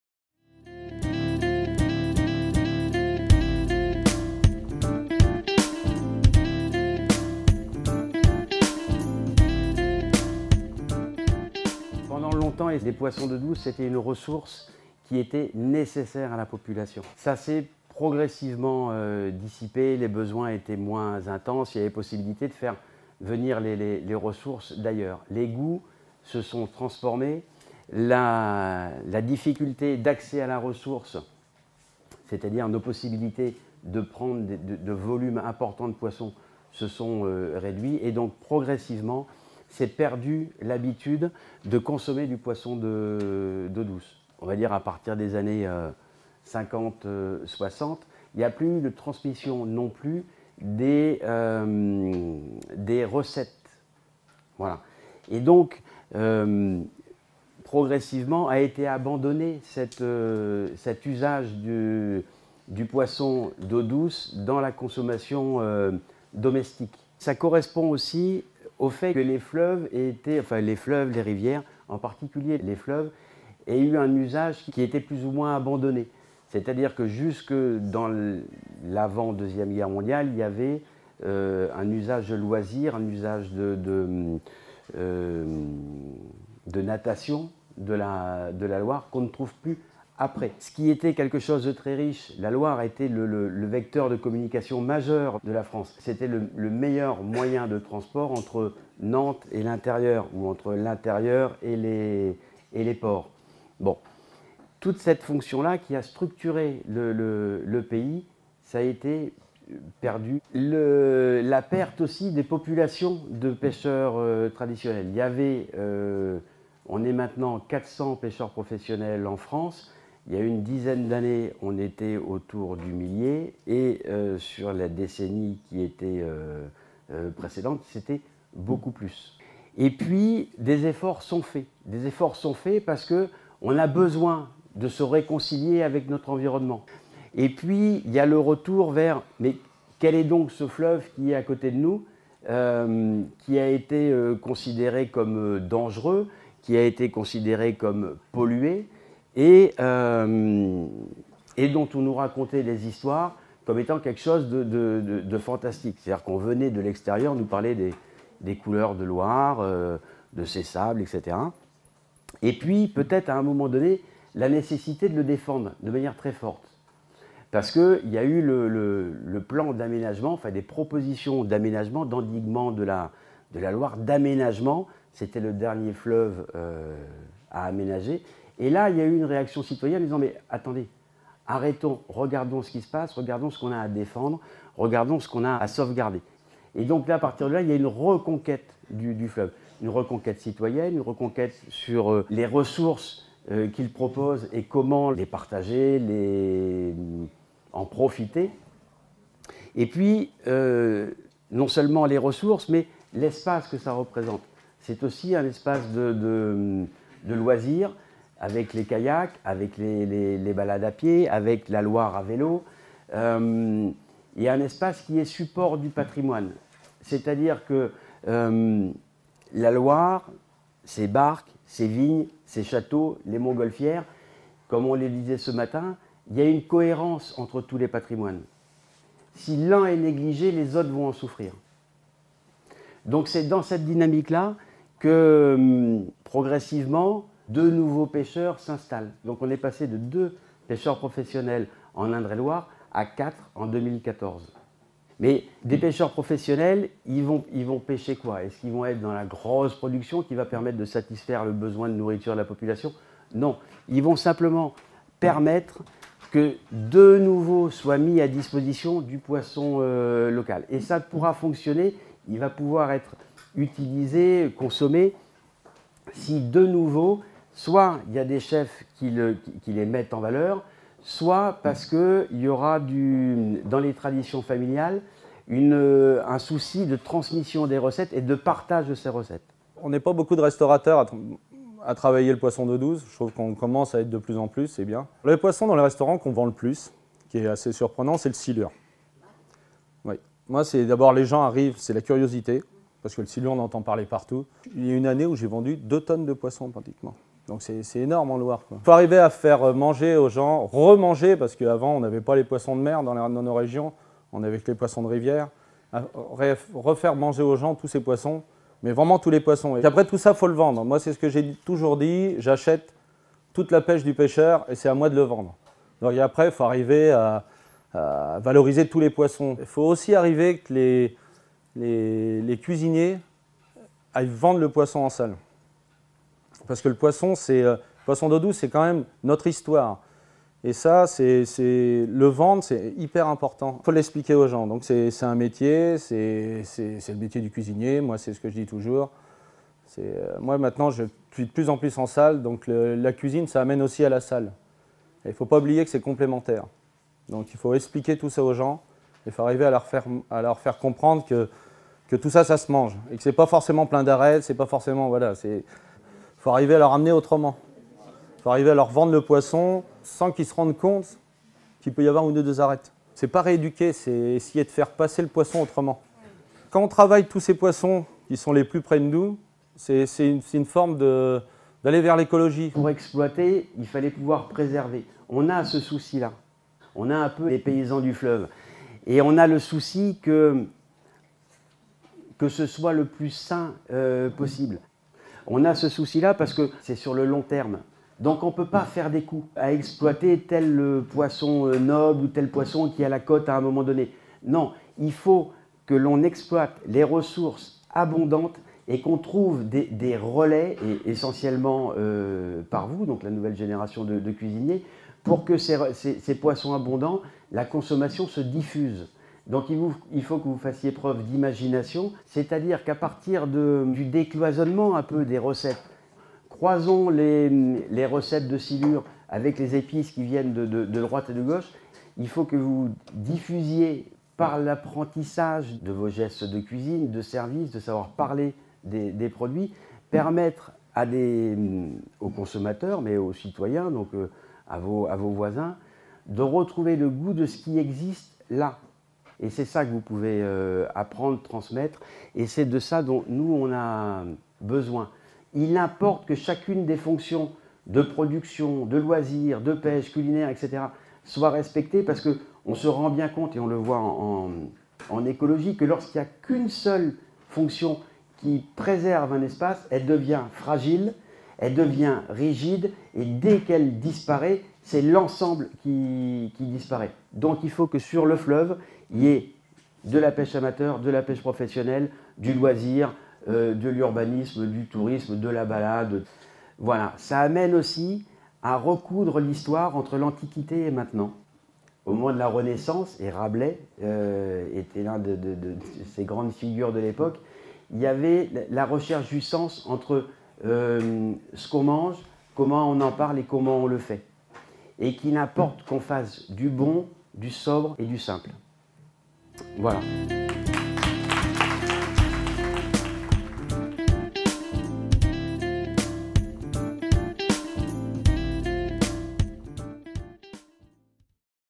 EFES | La reconquête du fleuve comme espace de production et la cuisine des poissons d’eau douce
Par Thierry Bouvet pêcheur professionnel et Florent Martin chef du restaurant le Martin Bleu.

Conférence présentée par l'IEHCA et le CFA de la Ville de Tours, lors des conférences des rencontres européennes de la gastronomie qui ont eu lieu à Tours, en mars 2017, dans le cadre du projet européen EFES.